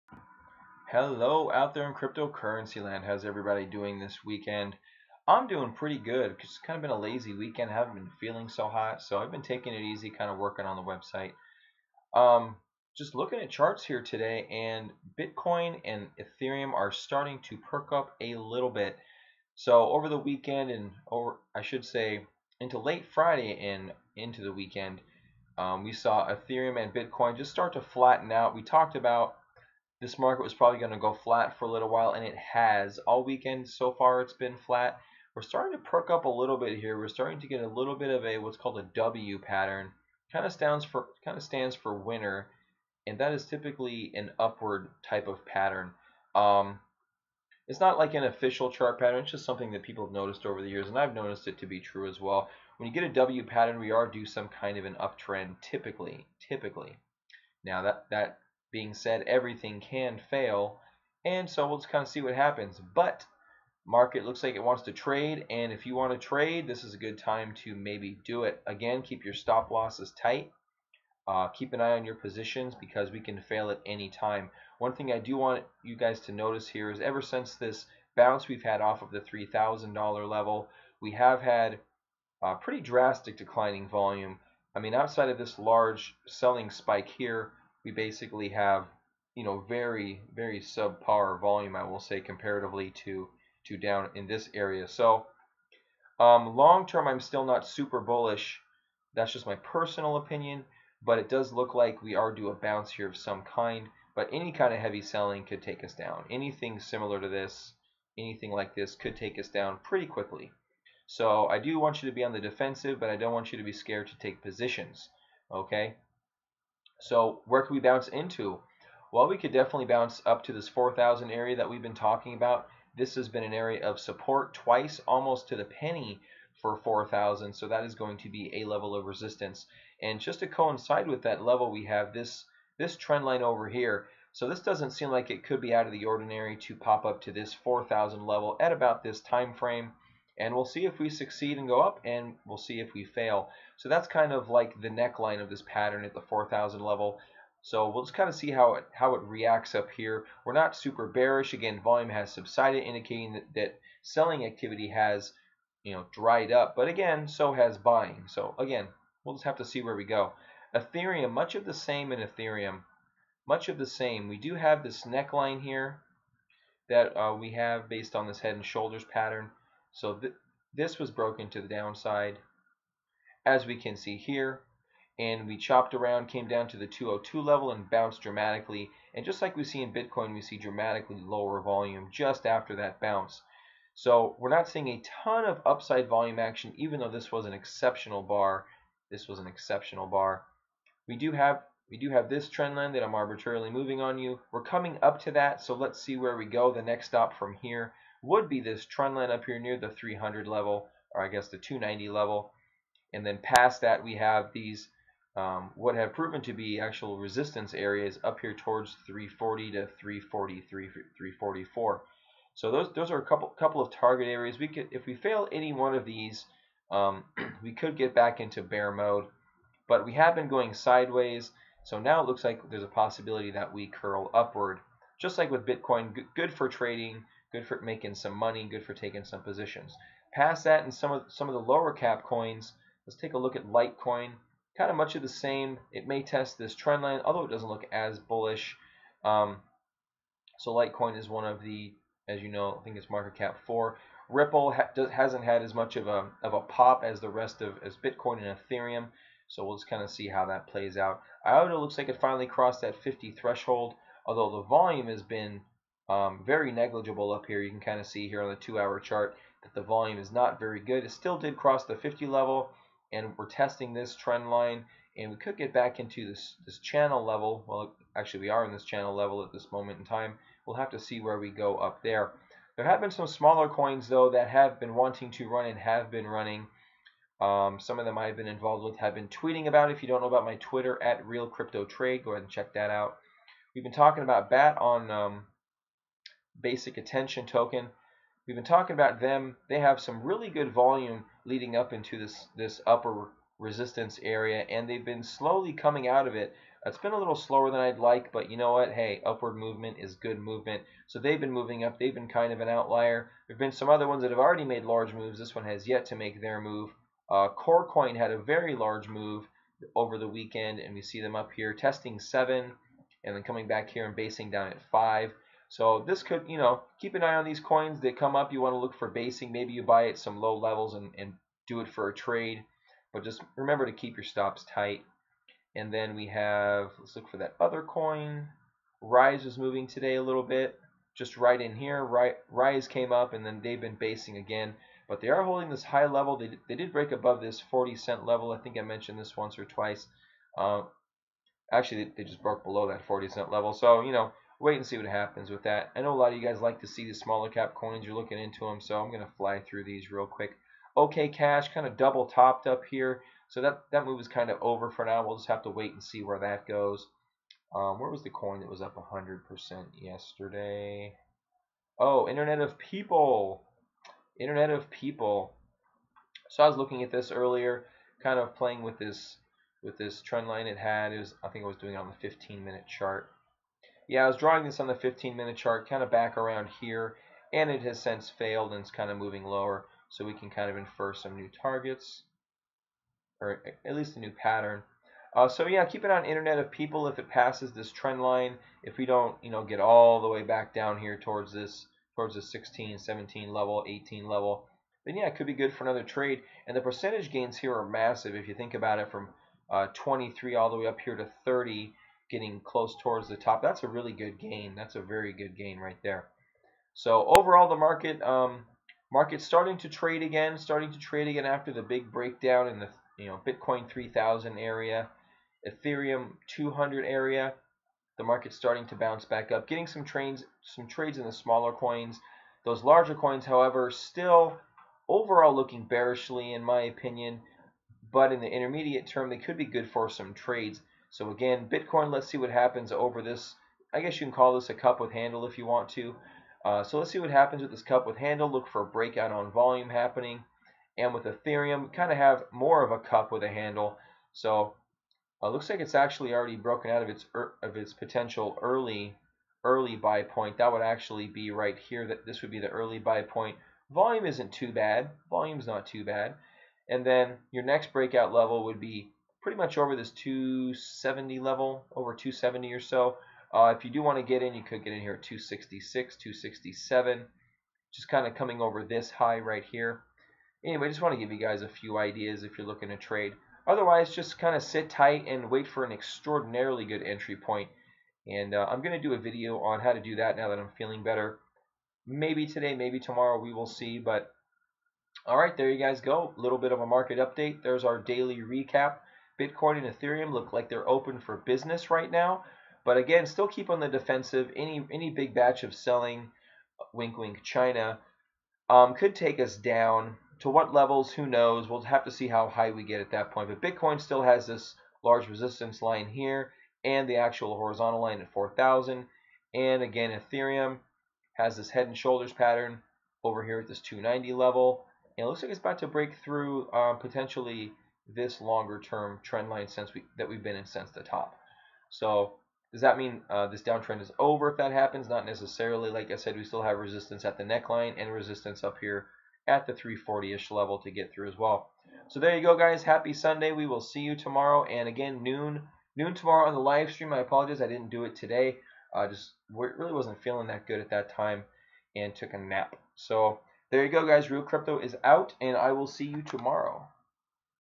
Bitcoin market update 9-17-2017
Bitcoin update for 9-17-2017, the market looks like it may be open for some trading so take advantage and keep stops tight.